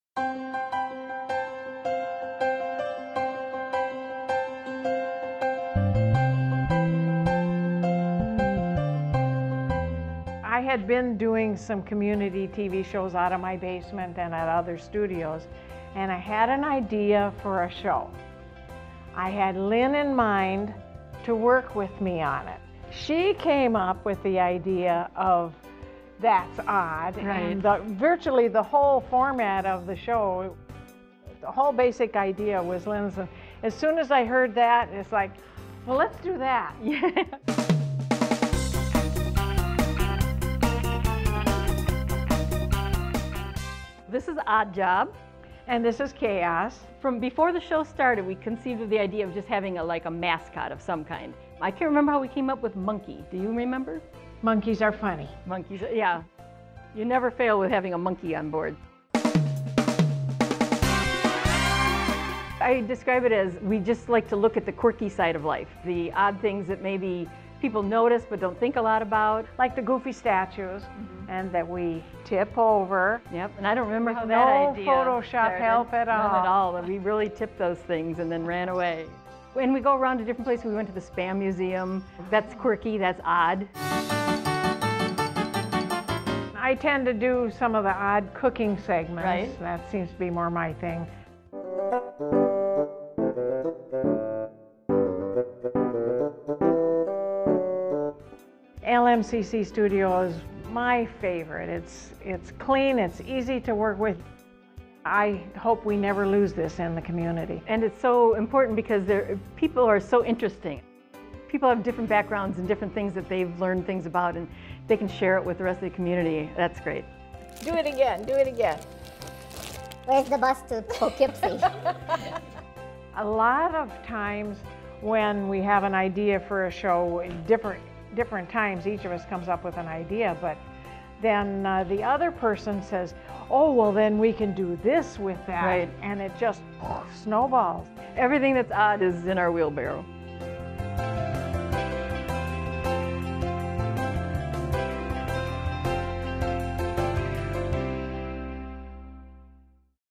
2019 LMCC Volunteer Producers of the Year: Lynn Garthwaite & Nadia Giordana
Each year, LMCC recognizes a volunteer who has done an outstanding job producing programming for the community...

We're pleased to announce that Lynn Garthwaite and Nadia Giordana are LMCC's 2019 Volunteer Producers of the Year!

Lynn and Nadia are the masterminds behind "That's Odd", a show that spotlights the quirky side of life.

Congratulations Lynn and Nadia!